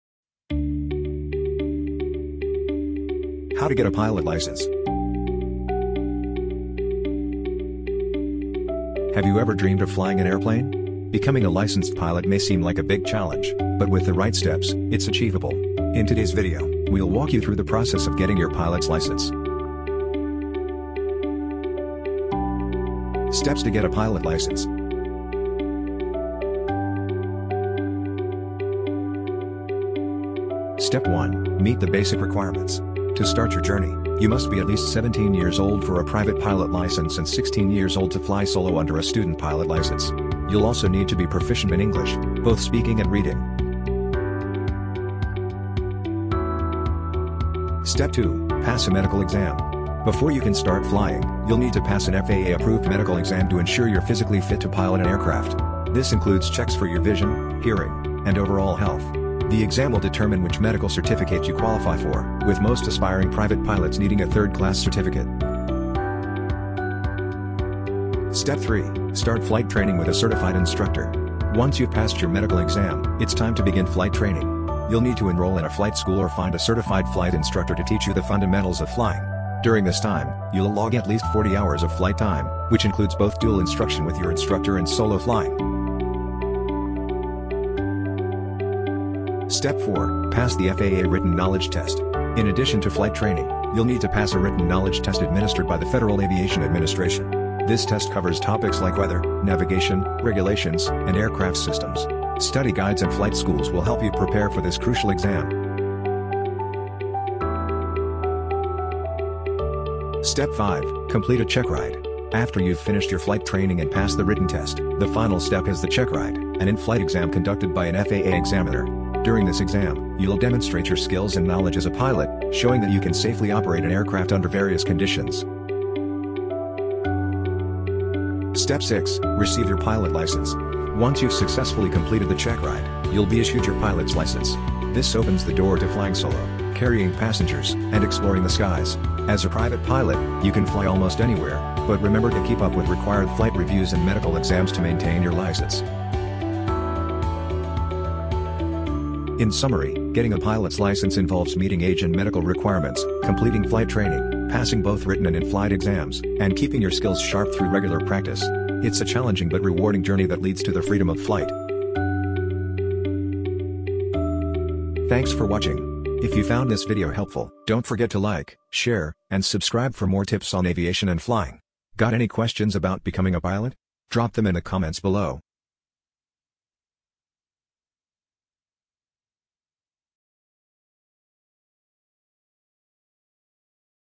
How to Get a Pilot License?
To get a pilot license, you need to follow a few key steps. First, you must meet the basic requirements, such as being at least 17 years old for a private pilot license and passing an FAA-approved medical exam. Next, you’ll enroll in flight training with a certified instructor, where you'll log at least 40 hours of flight time. After that, you’ll need to pass the FAA written knowledge test and complete a checkride, which is an in-flight exam with an FAA examiner. Once you’ve passed these steps, you’ll receive your pilot license and can take to the skies!